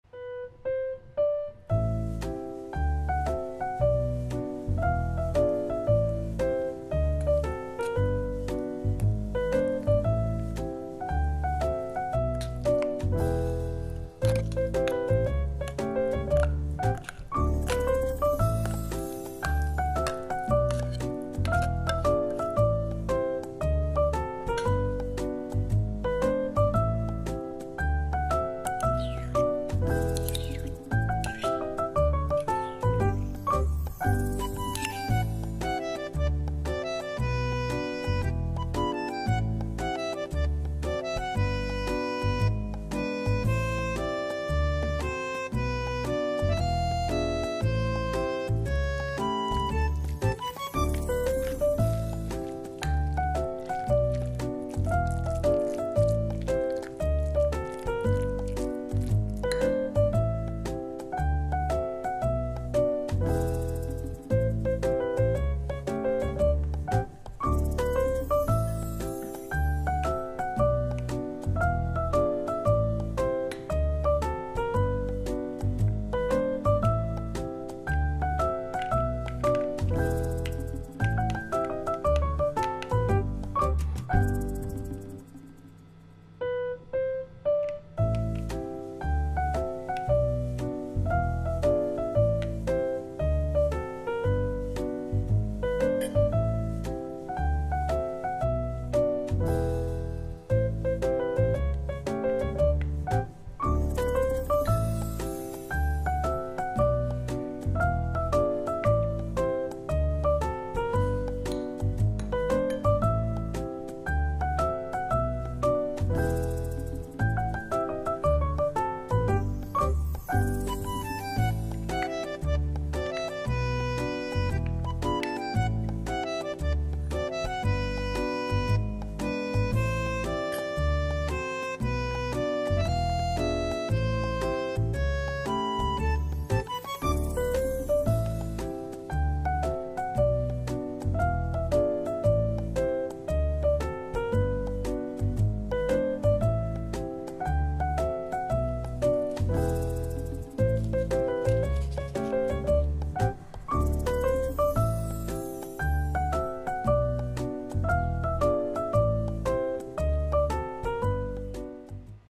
SUPER EASY TUNA MAYO ONIGIRI (JAPANESE TUNA RICE BALL)
TUNA MAYO ONIGIRI
- 1 canned tuna
- 2 tbsp kewpie mayonnaise
-1/4 tsp black pepper and garlic powder
- Steamed white rice
- Onigiri mold
- Nori (Roasted Seaweed) cut into small strips